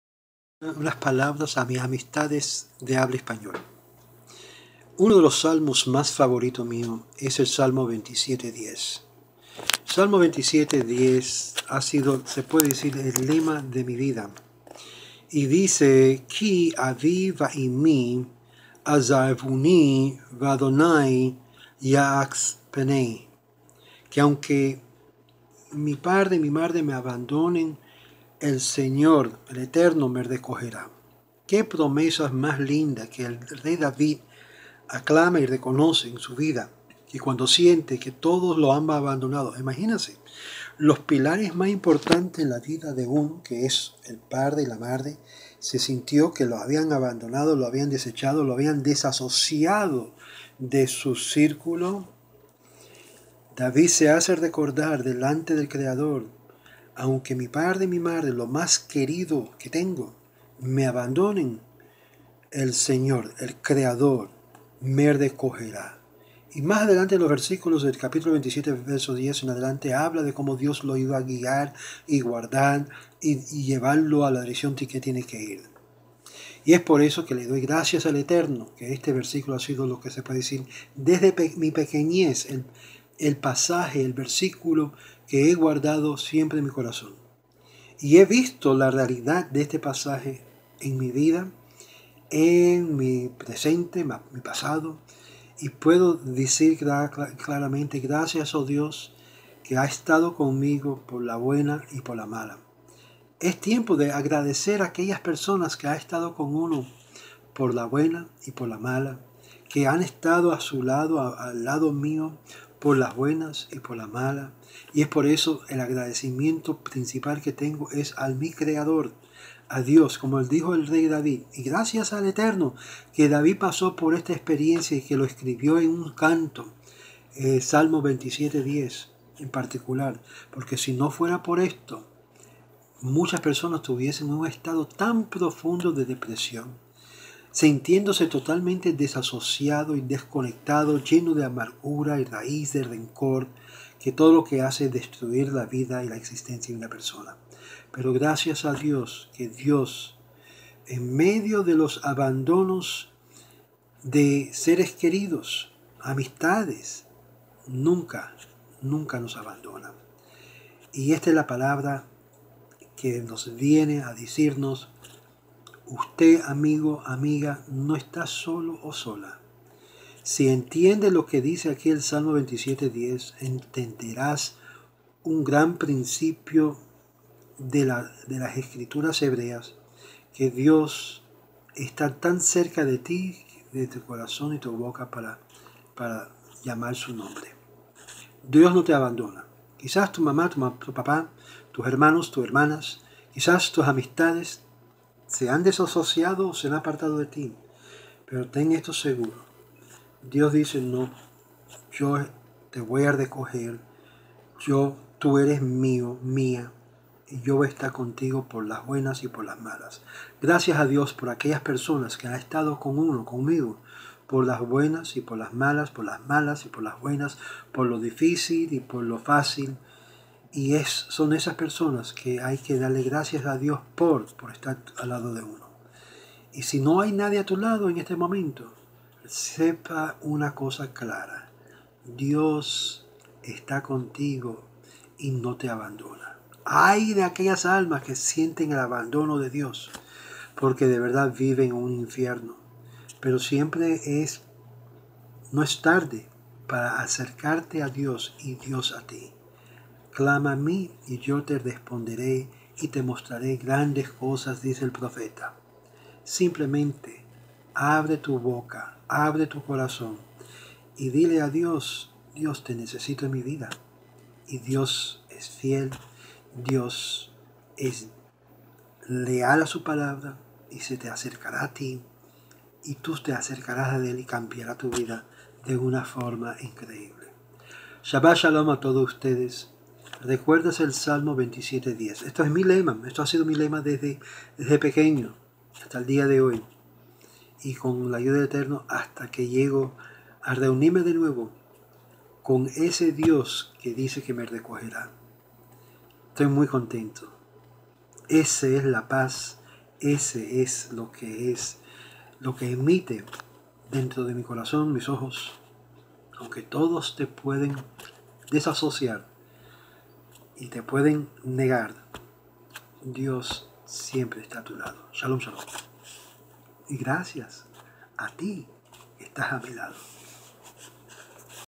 Salmo/Tehillim 27:10...Te sientes abandonado(a)?
El Salmista David entendio el secreto cuando los seres querido...familiares...mas a llegado le abandona o se apartan de ti como si fuera una lepra...El Eterno promete recogerte...guiarte y estar contigo. Gracias a Ds por estar conmigo...y Gracias a todos que han estado con uno por las malas y las buenas. Ds nunca nos abandona.